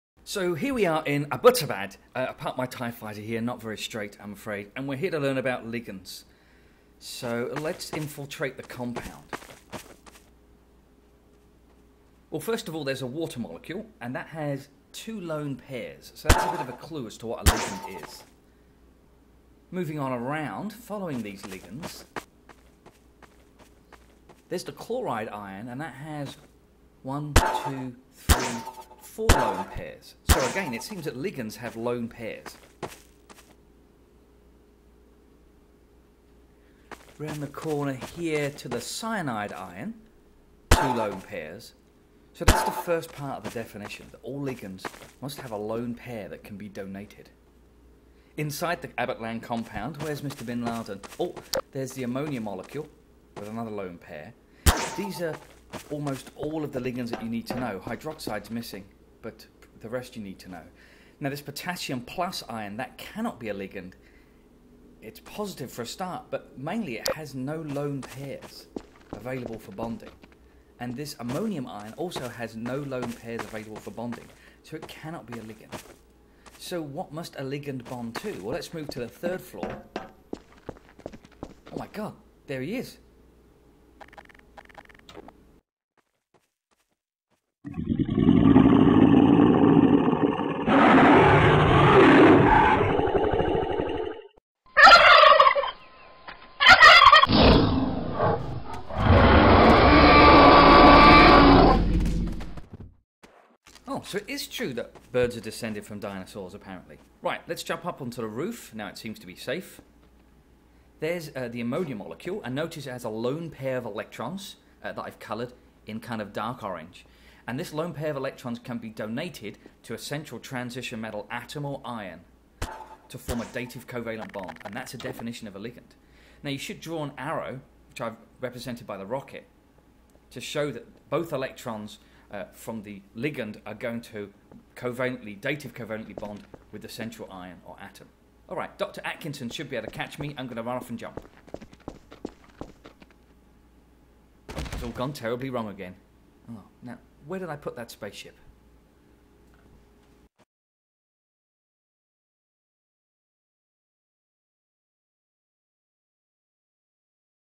BONUS: Define the term ligand (in Bin Laden's compound) [HL IB Chemistry]
Ligands are a species (atom or ion) that can donate an electron pair to a central transition metal atom/ion to form a dative covalent bond.
Common ligands include H2O, CN-, OH- NH3 Cl-.
Ligands must have a lone pair but they do not have to be negative - they can be neutral. but never positive.

Dr Atkinson had his spine realigned and is currently looking for a "no win no fee" lawyer.